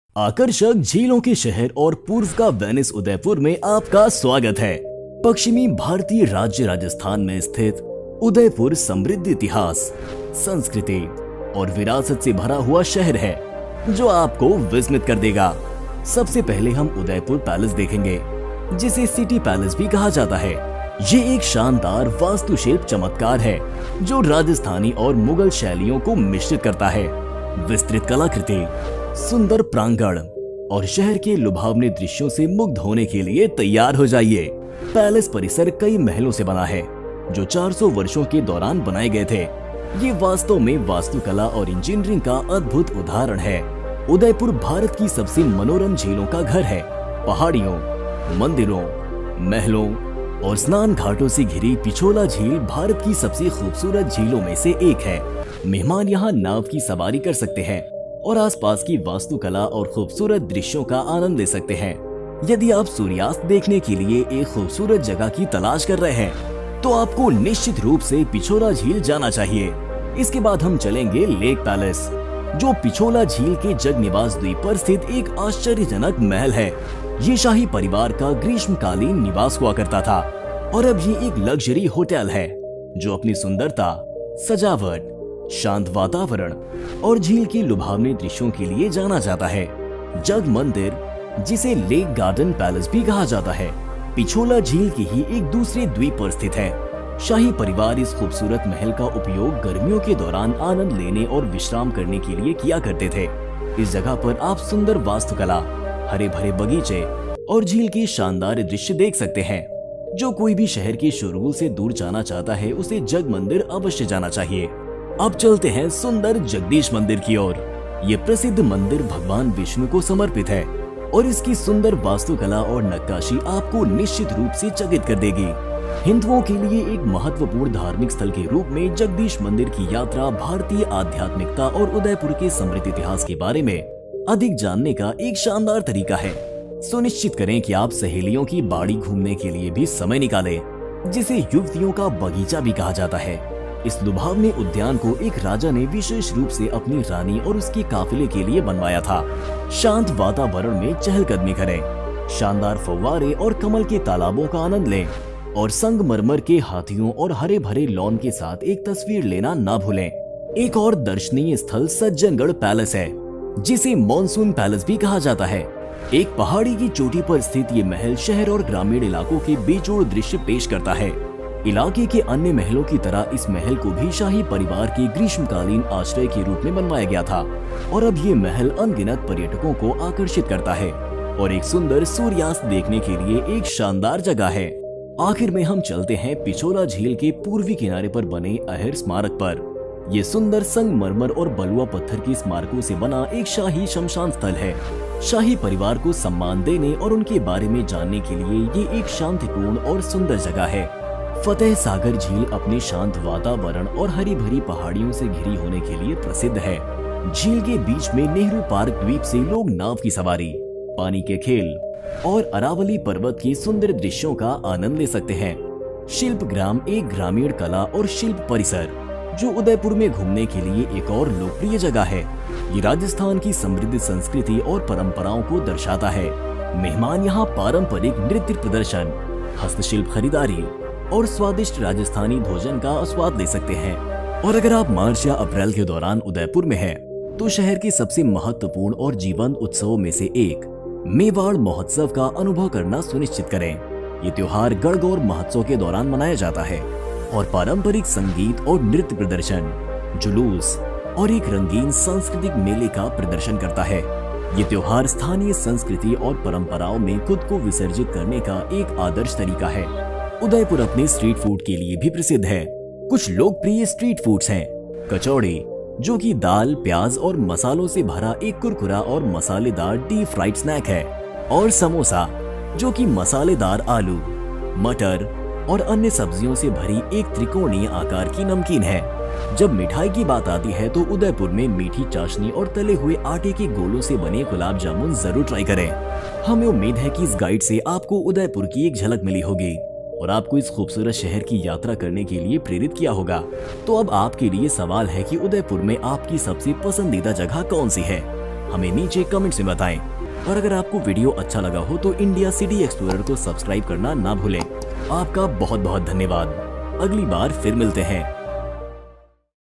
उदयपुर की खोज: करामाती 'झीलों के शहर' में घूमने के लिए सर्वश्रेष्ठ स्थान
उदयपुर राजस्थान की एक खूबसूरत जगह है। इसे "झीलों का शहर" और "पूर्व का वेनिस" कहा जाता है। अपने समृद्ध इतिहास, संस्कृति और विरासत के साथ, घूमने के लिए अद्भुत जगहों की कोई कमी नहीं है। उदयपुर पैलेस से सुरम्य झील पिछोला तक, आगंतुक आश्चर्यजनक वास्तुशिल्प चमत्कारों का पता लगा सकते हैं और शांत नाव की सवारी का आनंद ले सकते हैं। लेक पैलेस, जगमंदिर, जगदीश मंदिर, सहेलियों की बाड़ी, और सज्जनगढ़ पैलेस अन्य दर्शनीय स्थल हैं। शिल्पग्राम परिसर में, आप पारंपरिक राजस्थानी कला और शिल्प को देख सकते हैं। आप स्वादिष्ट स्ट्रीट फूड जैसे कचौरी, समोसा और गुलाब जामुन भी खा सकते हैं। यदि आप मार्च या अप्रैल में वहां हैं, तो जीवंत मेवाड़ महोत्सव देखने का मौका न चूकें। उदयपुर में हर किसी को अपनी पसंद का कुछ न कुछ मिल सकता है।

Timestamps:

0:00 स्वागत है
0:15 उदयपुर पैलेस
0:43 पिछोला झील
1:05 लेक पैलेस
1:21 जगमंदिर
1:43 जगदीश मंदिर
2:03 सहेलियों की बाड़ी
2:27 सज्जनगढ पैलेस
2:49 अहर स्मारक
3:05 फतेह सागर झील
3:19 शिल्पग्राम
3:34 मेवाड़ महोत्सव
3:57 स्ट्रीट फूड
4:26 सारांश